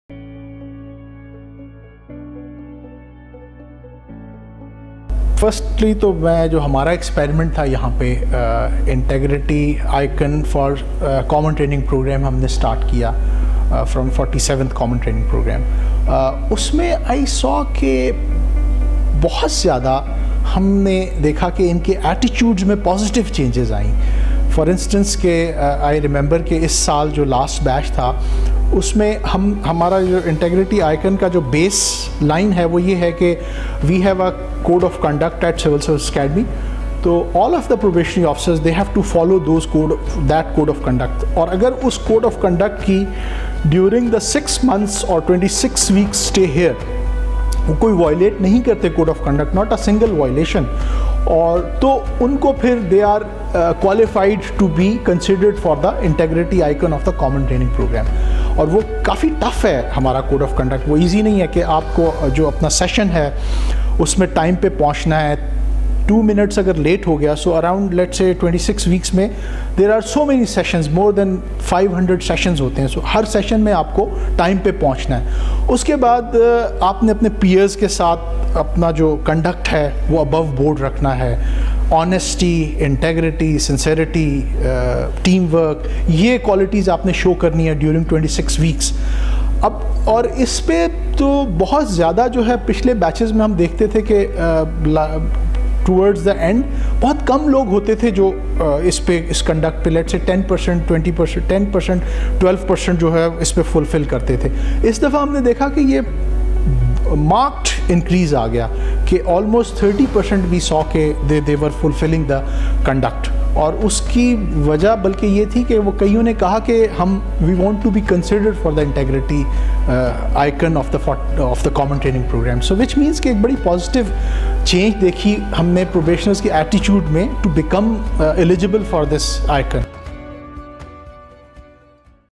Zulfiqar Sahib - Director Civil Services Academy
What is the role and importance of the Integrity Icon Campaign in bridging the trust gap between government departments and the general public?